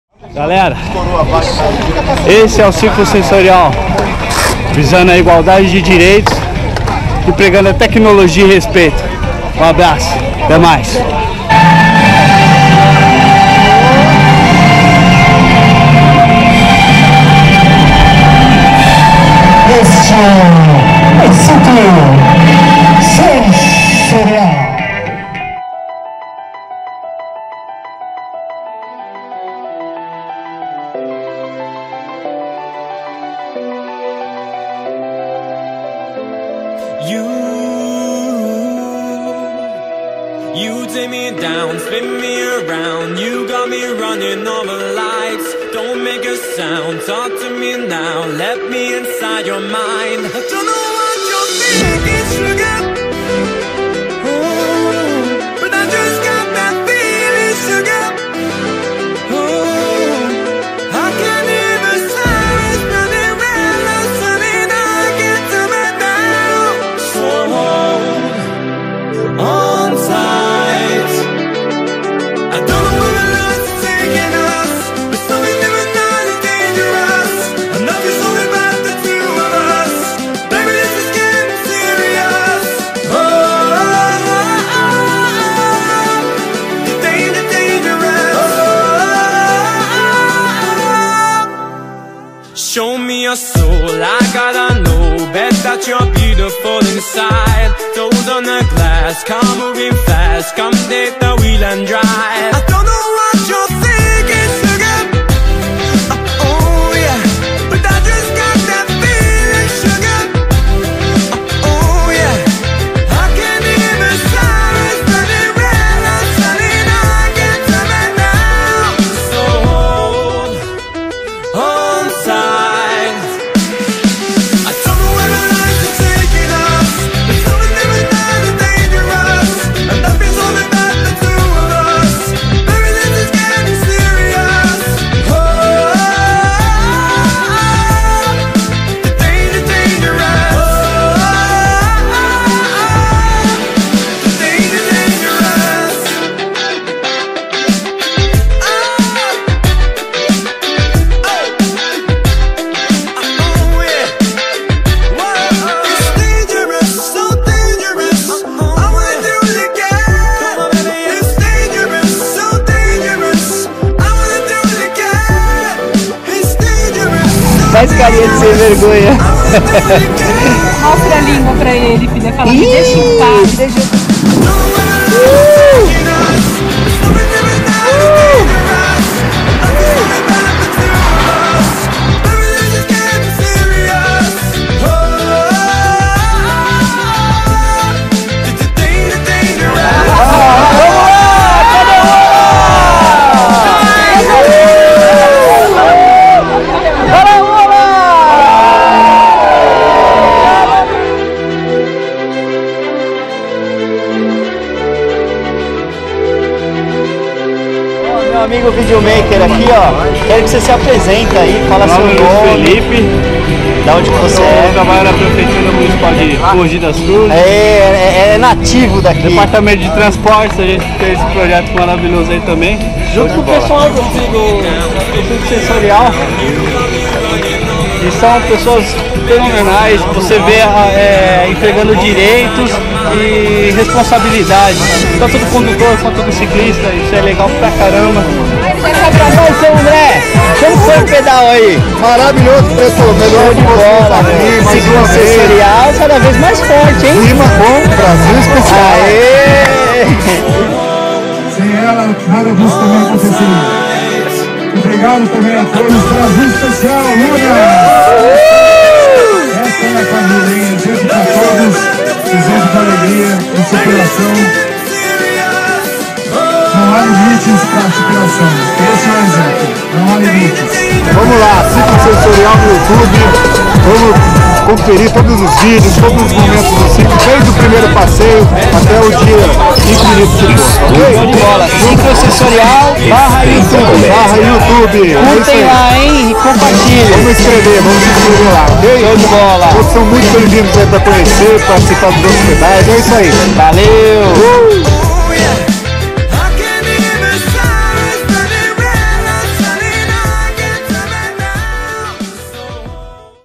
Brasil Especial Ciclo Sensorial Mogi 02 2016
Pedal em Mogi das Cruzes com a galera do Ciclo Sensorial.
Lindo trabalho com os deficientes visuais, físicos, auditivos e outros.
Andre Kuramoto e sua equipe do Ciclo Sensorial sempre proporcionando felicidade a todos!
Parabéns Ciclo Sensorial!